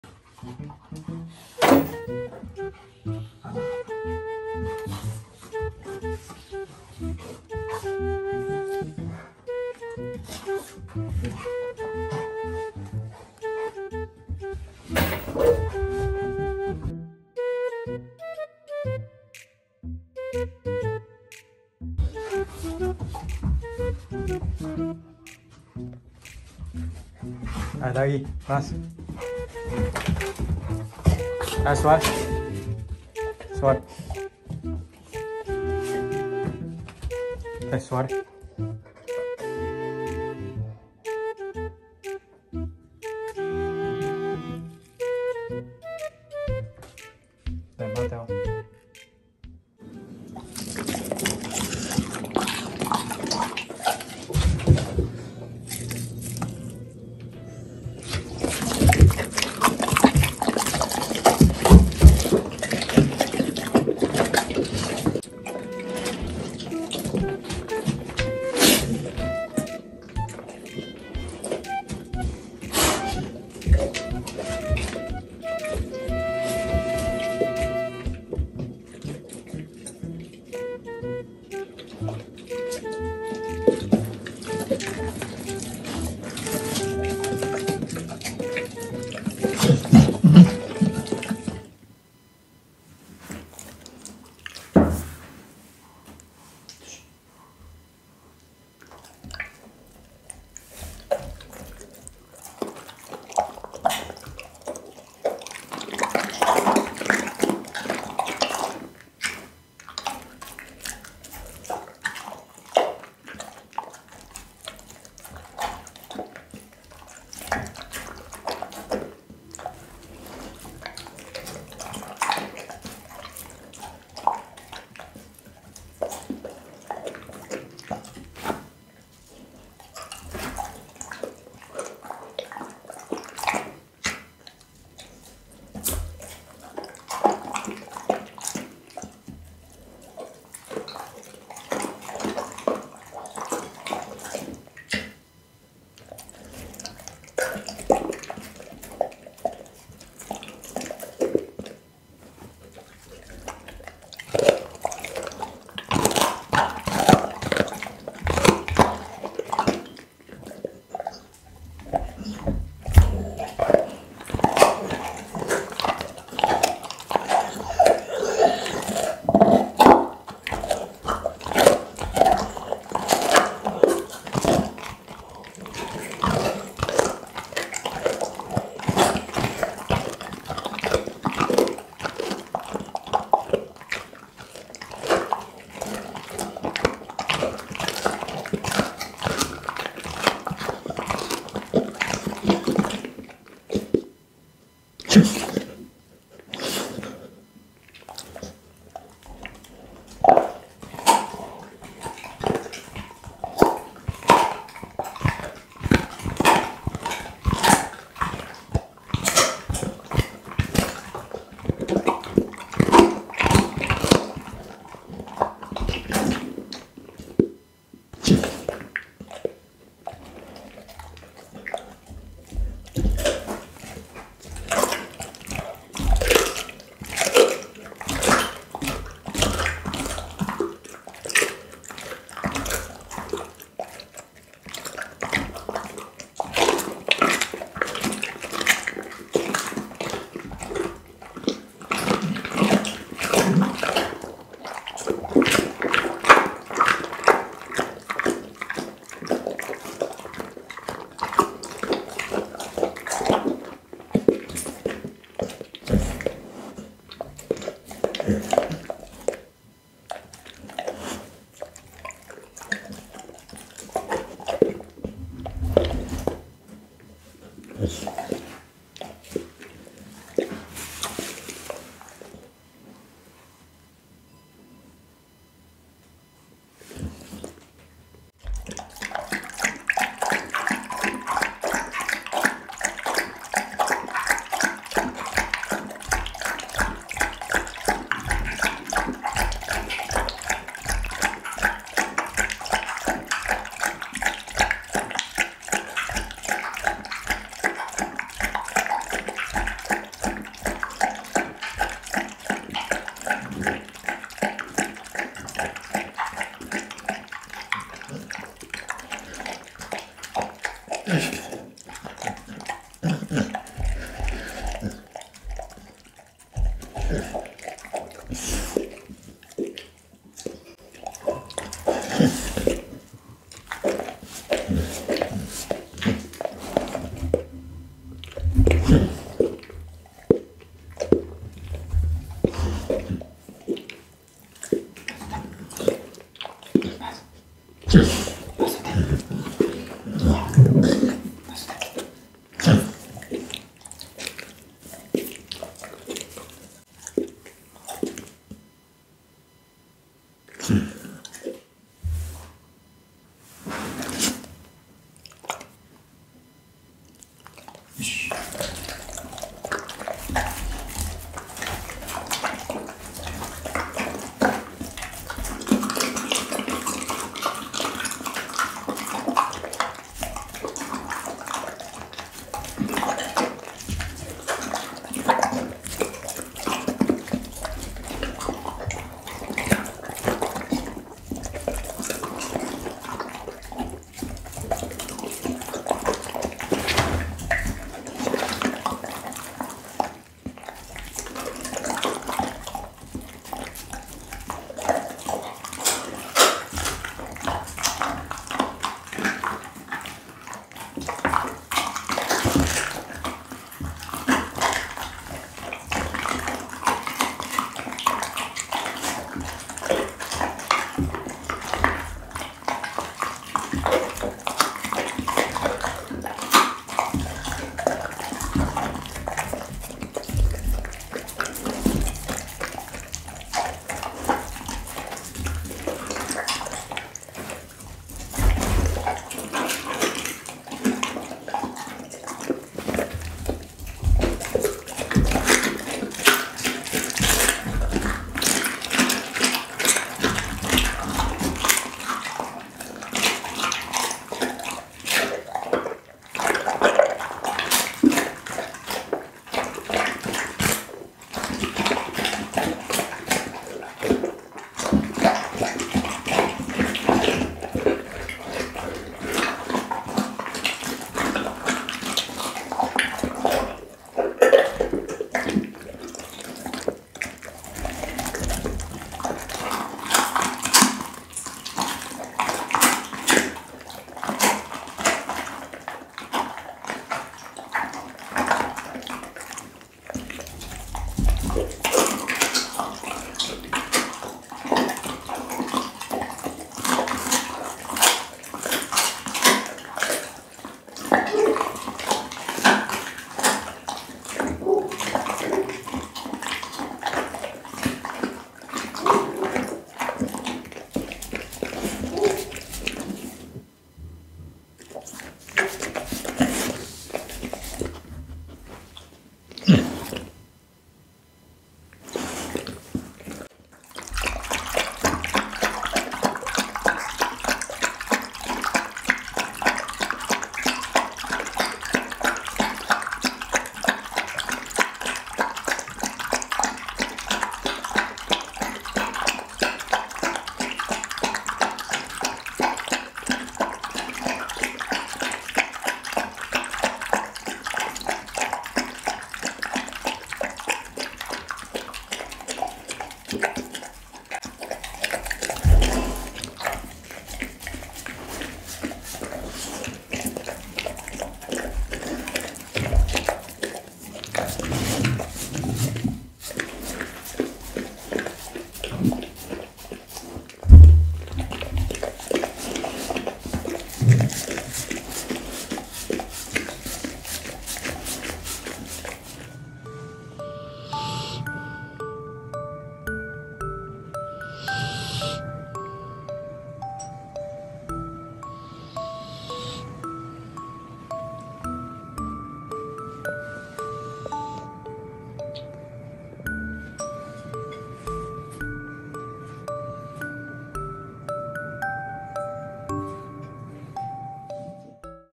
ASMR MUKBANG PITBULL EATING RAW FOODS DUCK HEADS PORK SPINE
Today's meal
Beef liver
Duck head
Pork stomach（boil）
Pork spine
Venison